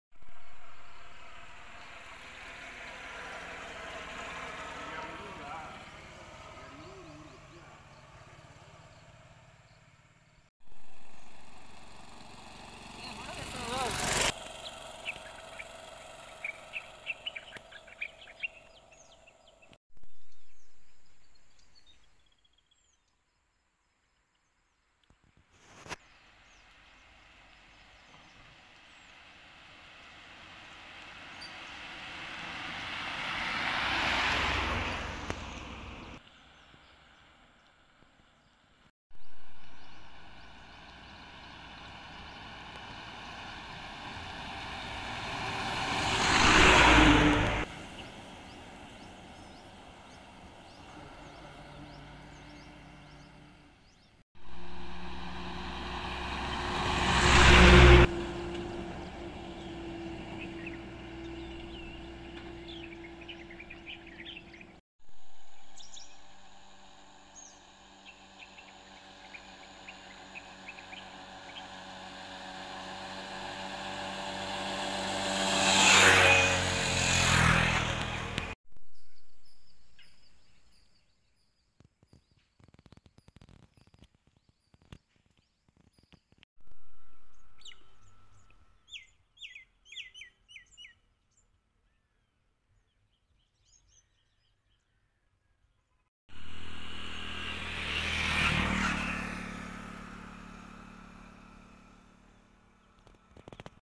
వరికుంటపాడు మండలంలో ఉదయం 8 గంటలకు కూడా మంచు వీడలేదు.
నెల్లూరు జిల్లా వరికుంటపాడు మండలంలో  ఉదయం 8 గంటలకు కూడా మంచు వీడలేదు.